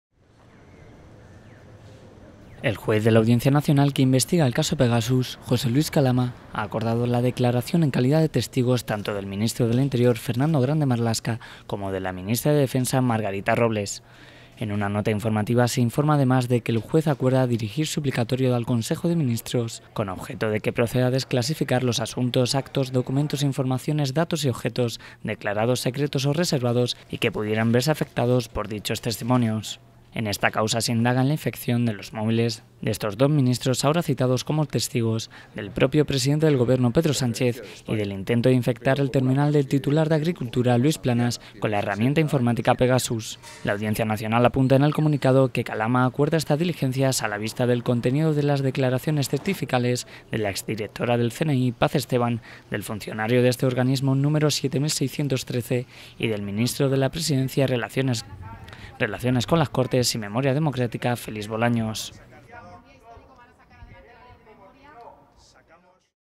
La Audiencia Nacional cita como testigos a los ministros Marlaska y Robles por 'Pegasus'
El juez de la Audiencia Nacional que investiga el 'caso Pegasus', José Luis Calama, ha acordado la declaración en calidad de testigos tanto del ministro del Interior, Fernando Grande-Marlaska, como de la ministra de Defensa, Margarita Robles.

(Fuente: EP/Congreso/Moncloa)